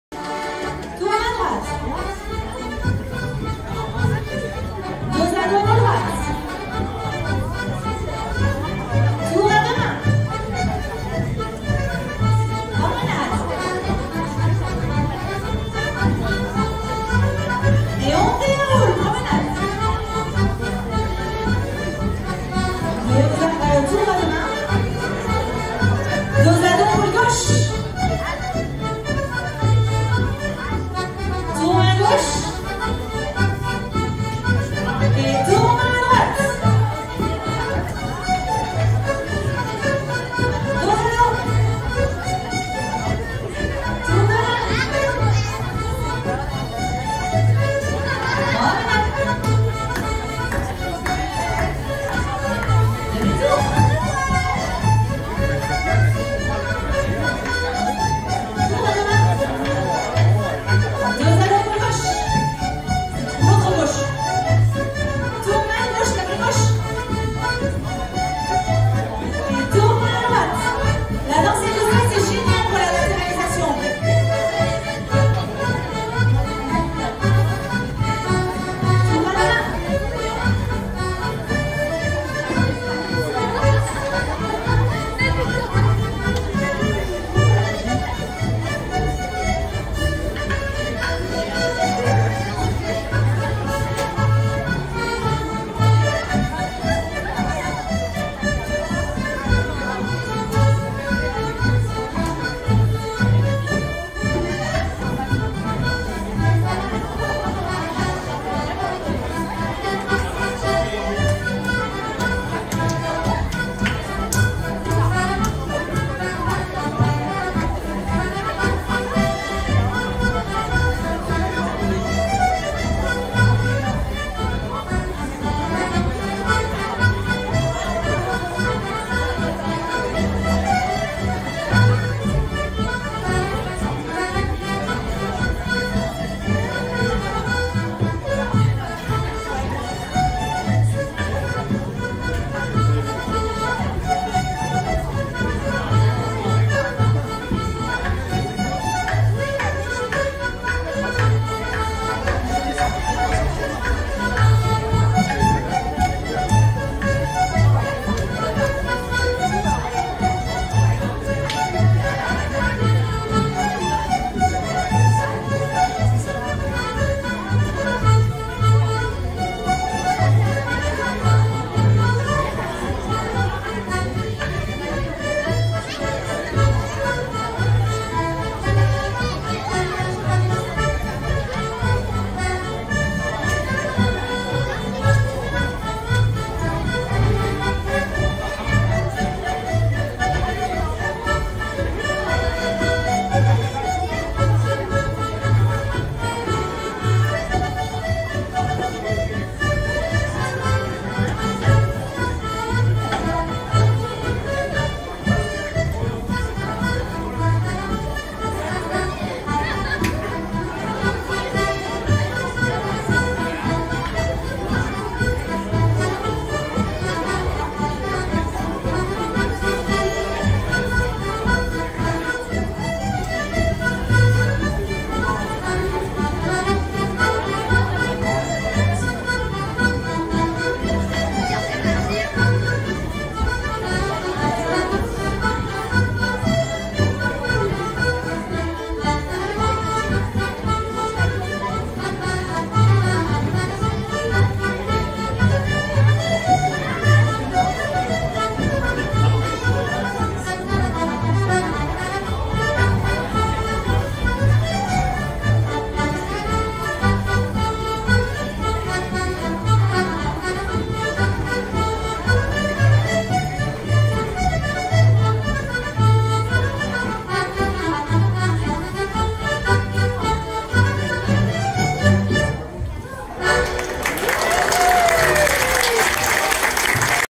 Le Palindrome
dansée lors du stage SCD organisé par le Méaudre Reel, mai 2022